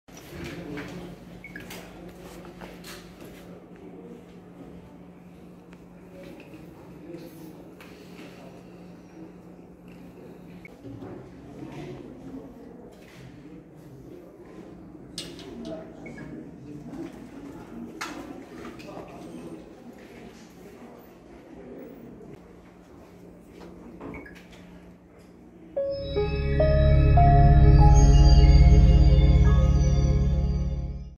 Vrau vëllanë dhe kunatën në Tiranë, Arsen Muskurti përshëndet mediat në sallën e gjyqit
reportTV lajmeshqip lajmeArsen Muskurti, autori i vrasjes së dyfishtë në Tiranë do të dalë sot në orën 10:00 para Gjykatës Penale për tu njohur me masën e sigurisë.